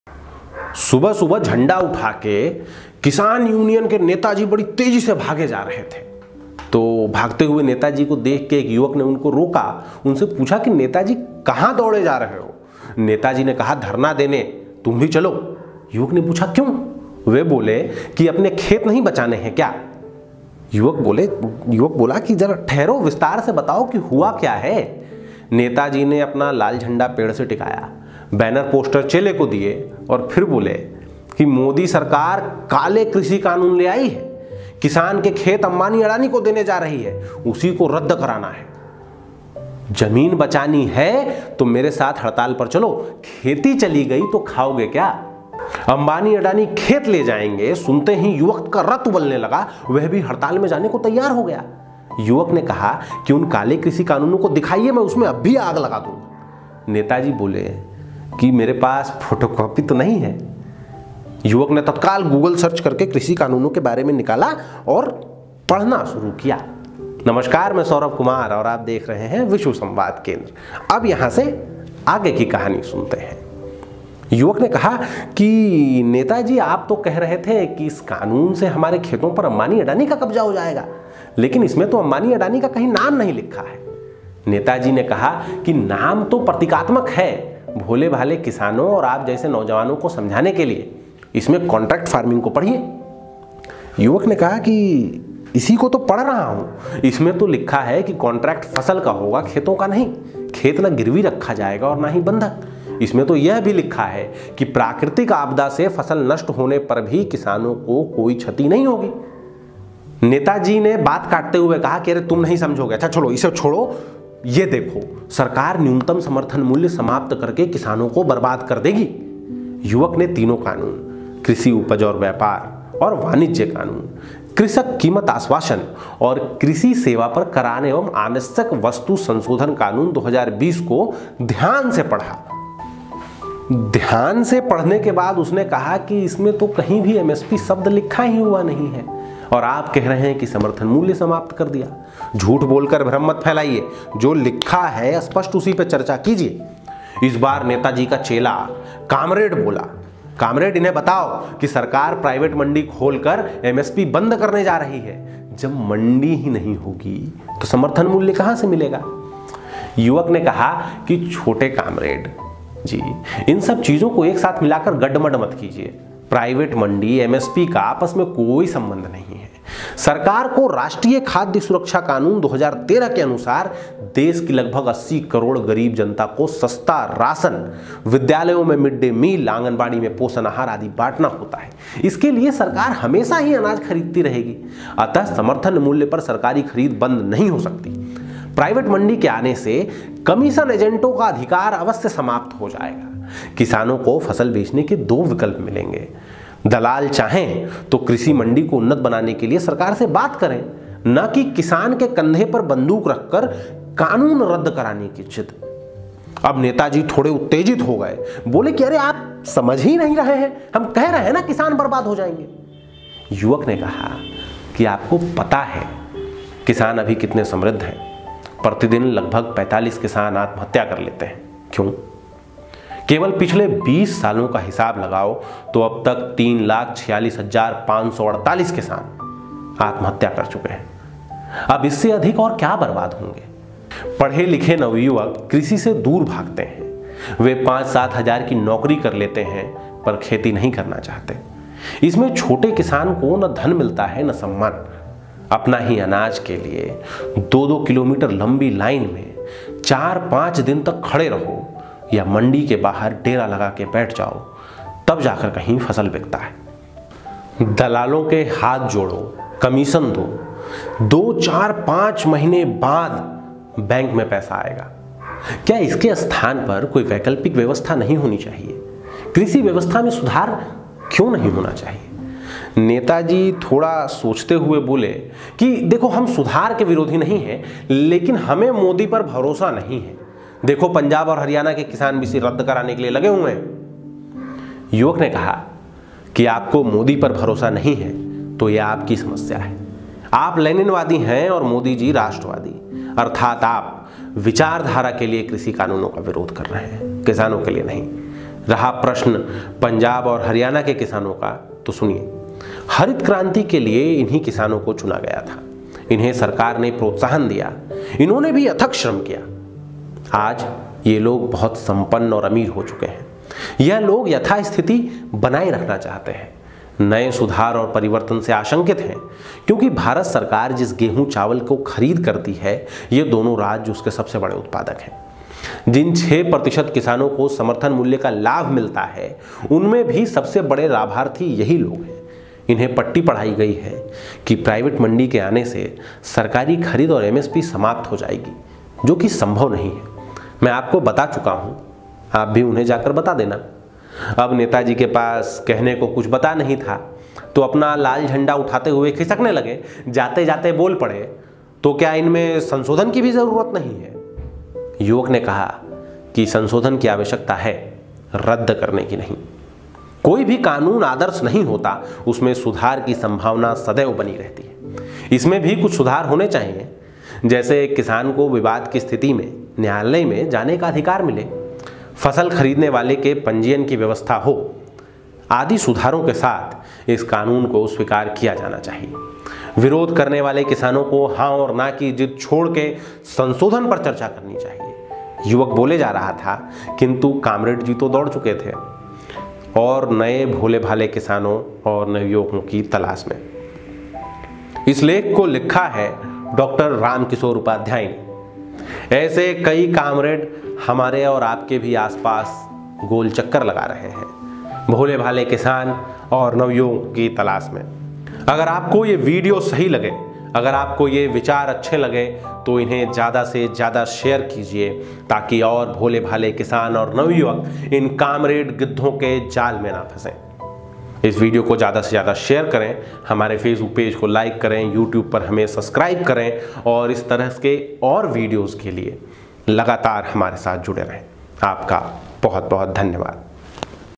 कृषि कानूनों की वामपंथी व्याख्या
कृषि कानूनों का विरोध करने के लिए कामरेड नेताजी लाल झंडा लेकर सुबह सुबह निकले थे की उन्हें रास्ते में एक पढ़ा लिखा युवक मिल गया.
सुनिए उसके बाद क्या हुआ ?